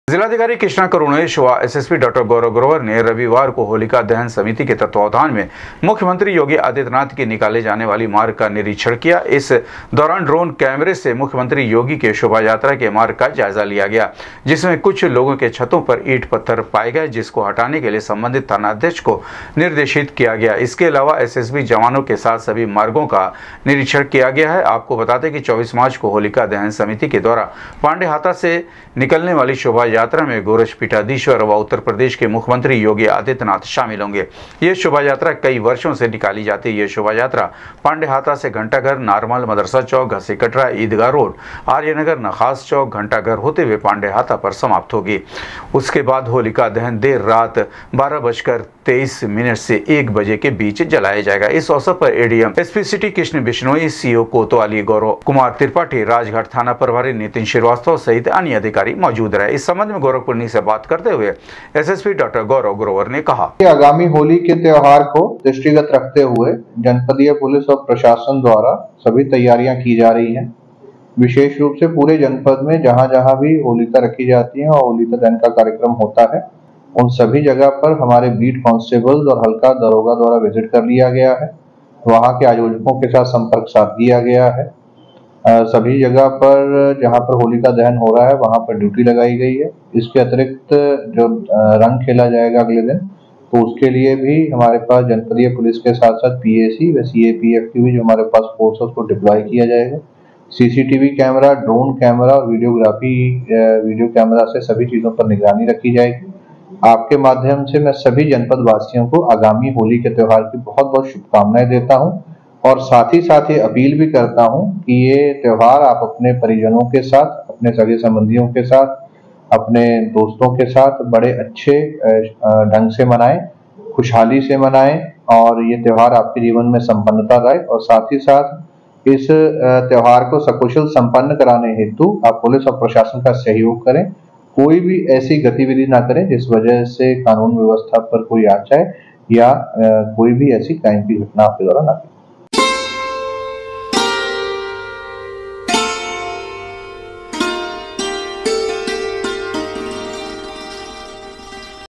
मुख्यमंत्री योगी के शोभायात्रा रूट का जिलाधिकारी व एसएसपी ने सुरक्षा व्यवस्था का लिया जाएजा
मुख्यमंत्री योगी के शोभायात्रा रूट का जिलाधिकारी व एसएसपी ने सुरक्षा व्यवस्था का लिया जाएजा,कहा शकुशल सम्पन्न होगा होली का पर्व,आरजकता फैलाने वालों पर होगी कार्यवाही